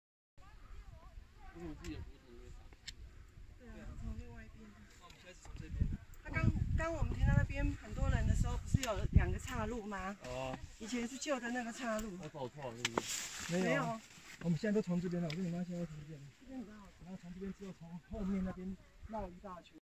陽明山-擎天崗.擎天崗大草原
擎天崗，日治時期稱大嶺峠，1934年日本人在此設立「大嶺峠牧場」，放牧牛等動物，範圍含現今擎天崗、冷水坑、七股山一帶。目前擎天崗草原主要由類地毯草及假柃木等組成，據稱最早即為日本人種植。
擎天崗草原又稱太陽谷，其位於大屯山群彙的中央，及竹子山、七股山與頂山，磺嘴山之中間按部，地勢平坦，源於竹篙山熔岩所形成的階地。日據時期日本人就在此設置大嶺峙牧場，作為種茶、放牧用途。台灣光復後政府依據原大嶺峙牧場中擎天崗、冷水坑一帶的規劃設置了陽明山牧場。